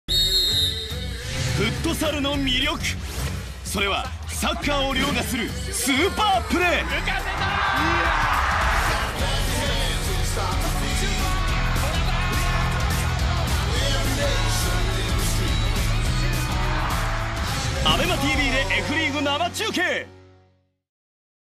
DUARIG Ｆリーグ2017/2018 第17節・第18節 AbemaTVシリーズをAbemaTVでライブ配信！
9月23日(土)、24日(日)に開催するDUARIG Ｆリーグ2017/2018 第17節・第18節 AbemaTVシリーズ べっぷラウンドをAbemaTVでライブ配信！

9月23日(土)　べっぷアリーナ
13:30　バサジィ大分 vs エスポラーダ北海道
16:00    府中アスレティックＦＣ vs デウソン神戸
18:30    バルドラール浦安 vs 湘南ベルマーレ

9月24日(日)　べっぷアリーナ
12:00　エスポラーダ北海道 vs デウソン神戸
14:30    バルドラール浦安 vs 府中アスレティックＦＣ
17:00    バサジィ大分 vs 湘南ベルマーレ